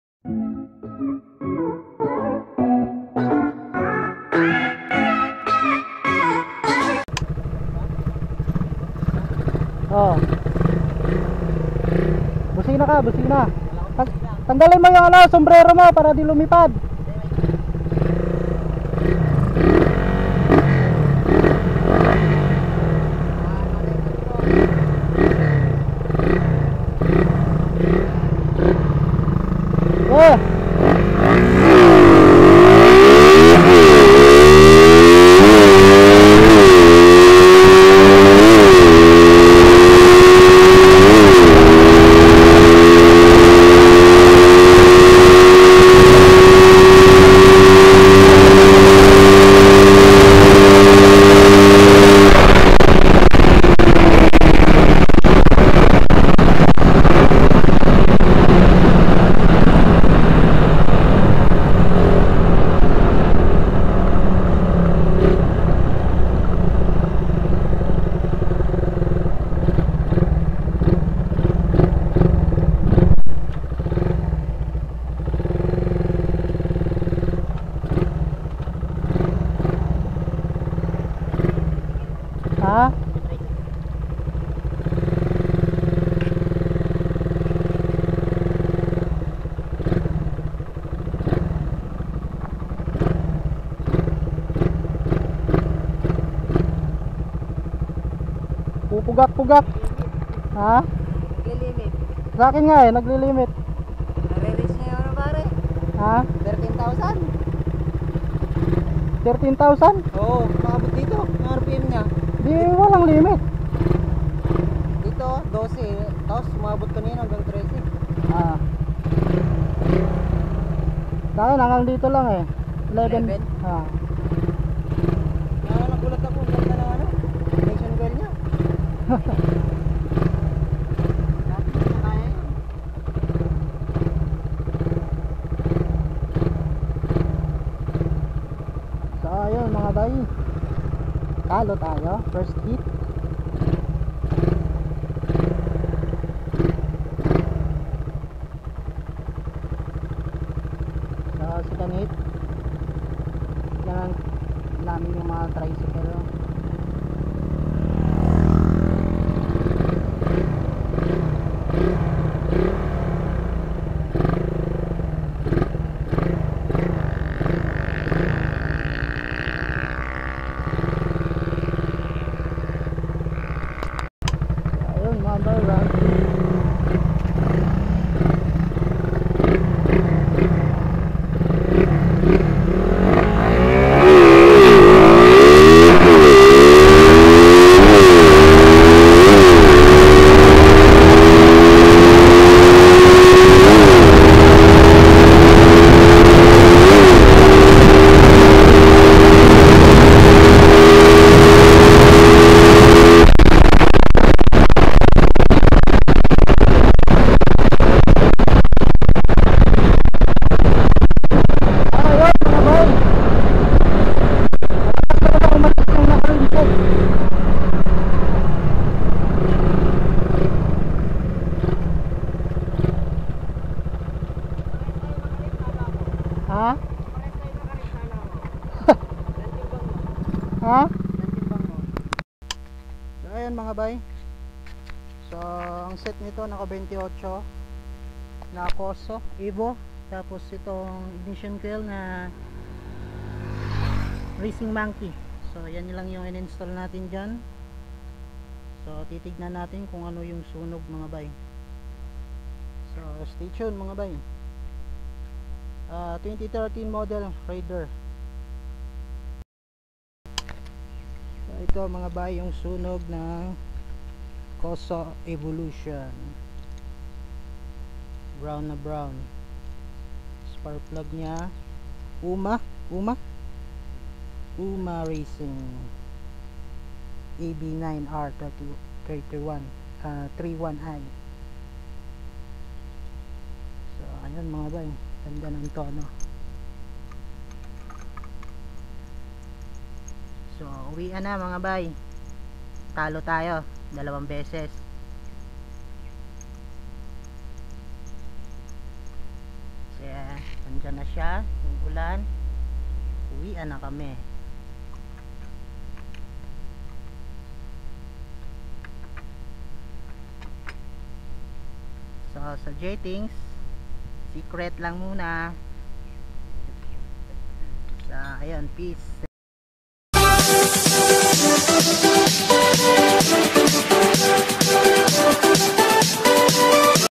raider 150 50kilos rider Vs raider 150 85 kilos rider lakas ng 2013 pinakain ako ng alikabok
Rim set 14 41combe 28mm carb racing manky ignition coil 50kilos rider
VS
Mag stock tire 30mm carb 14 43 combe
apido ignition coil 85 kilos rider